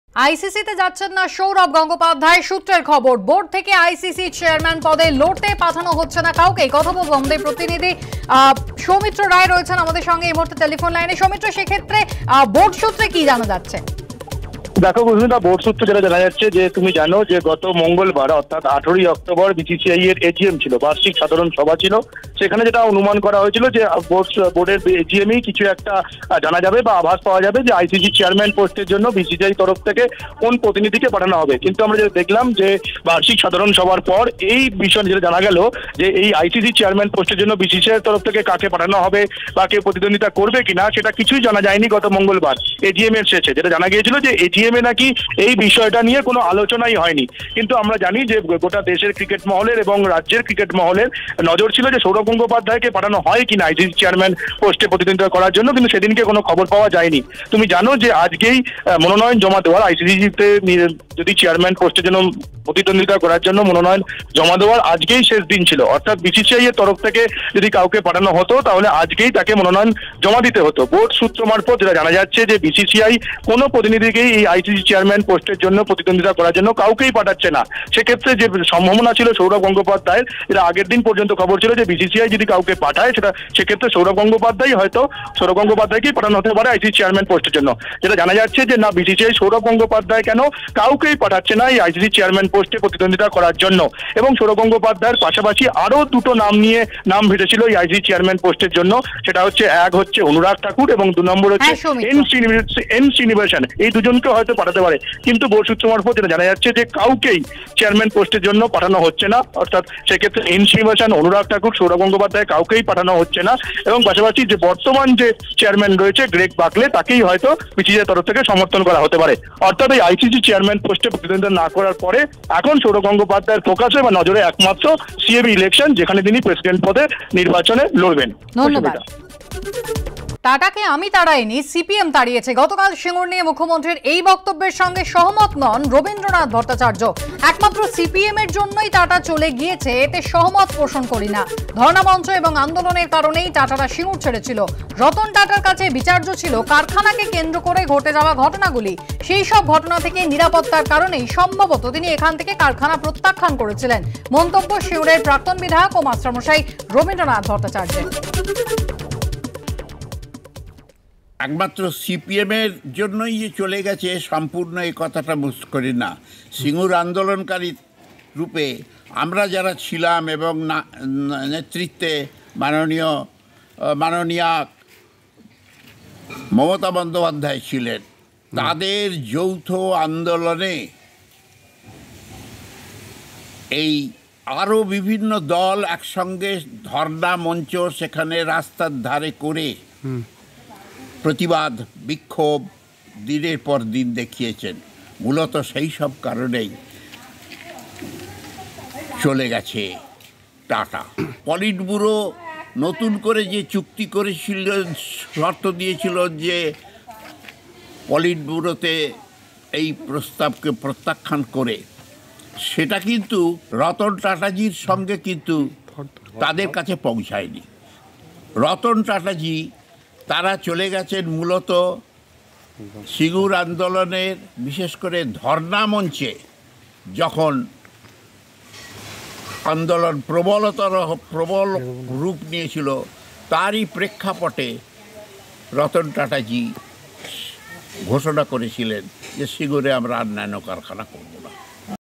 Sourav Ganguly: আইসিসি-র চেয়ারম্যান পদে মনোনয়ন জমা দিচ্ছেন না সৌরভ
আইসিসি-তে যাচ্ছেন না সৌরভ, খবর সূত্রের